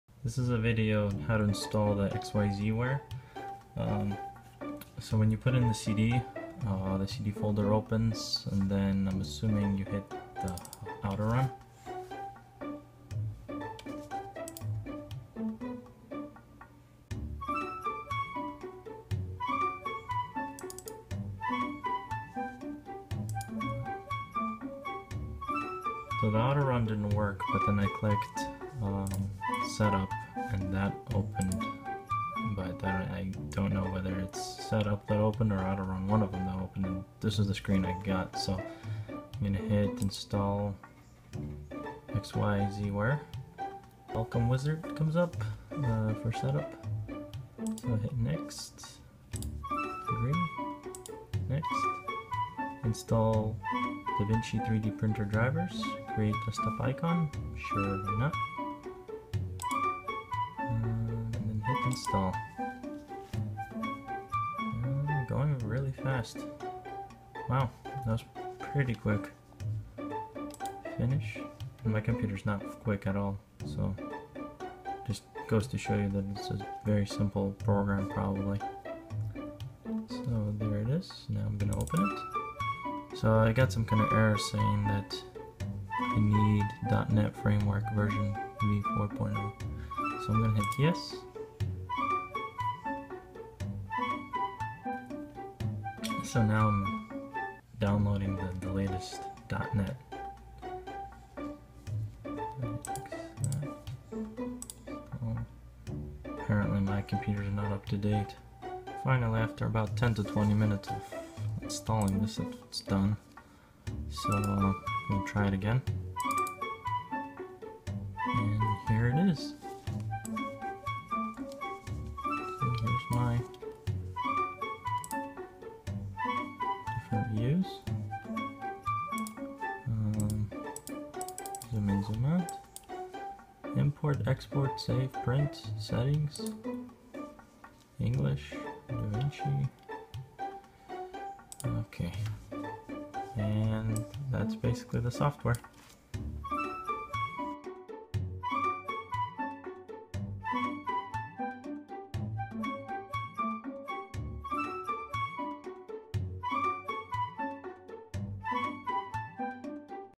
da Vinci 3D printer software XYZware install (.NET install also)
Installing the XYZware for da Vinci 3D Printer. I ran into some trouble because I didn't have the latest .NET installed. How to install XYZware software for da Vinci 3D Printer.

Here is where you can get your filament:

Here is the link where you can buy the 3D Printer: